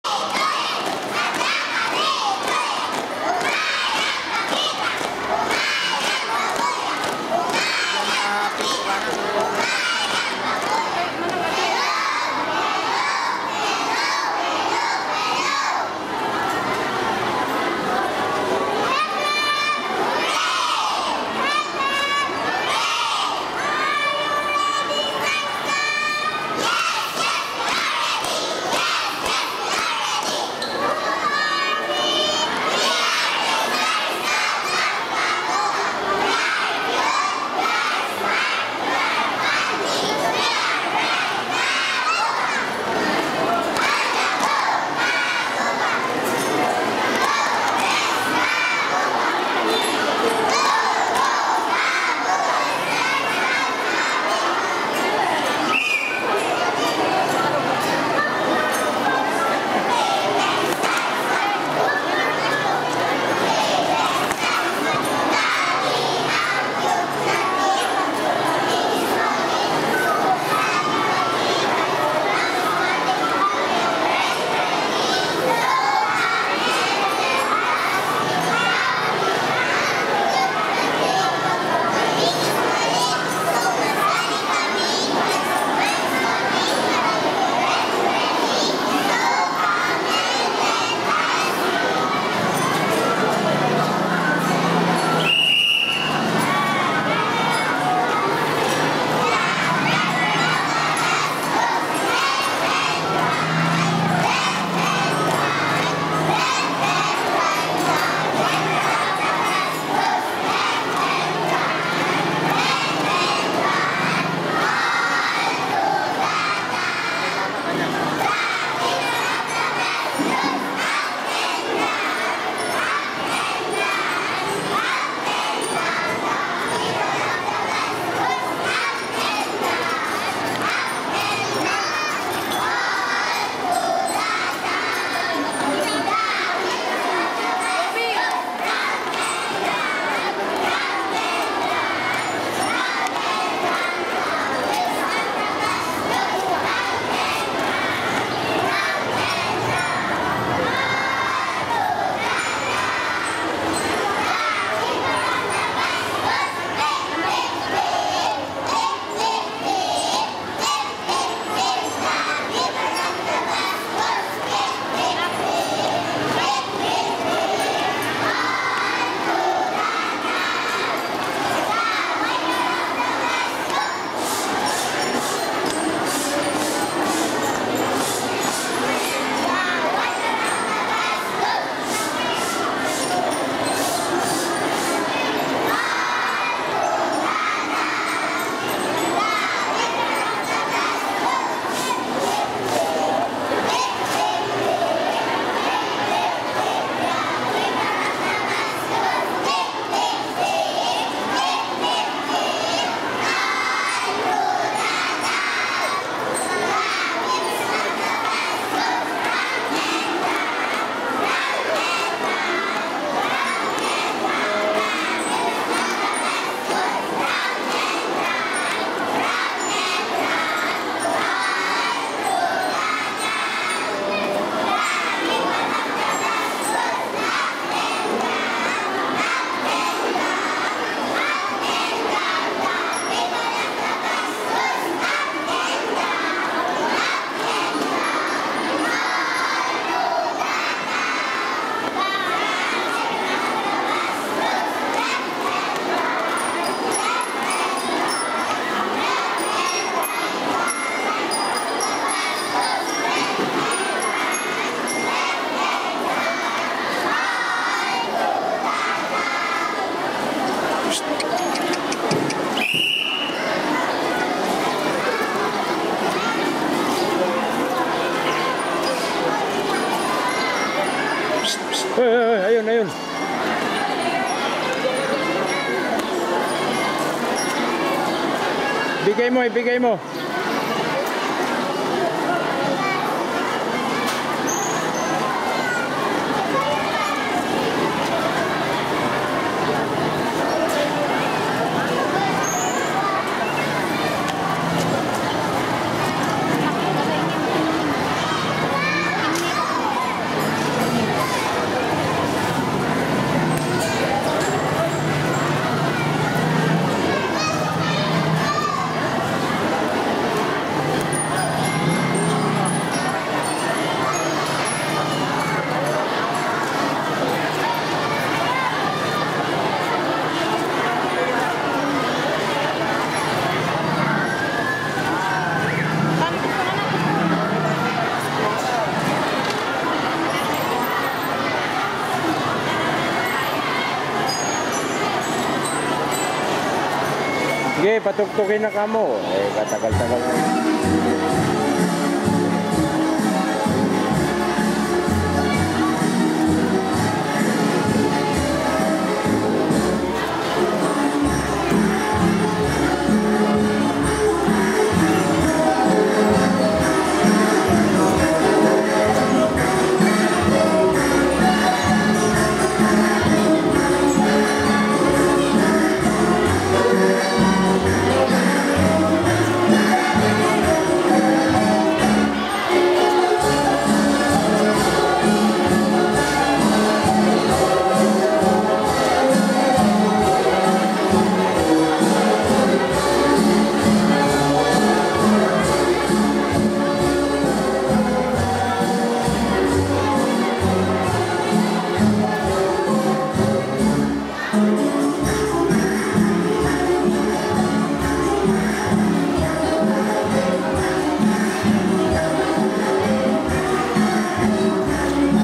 Napahanga ang mga JUDGES!!! 😁😁High level performance
STARS SCOUT... MEMORIES OF MY DAUGHTER WHEN SHE WAS AT PREP.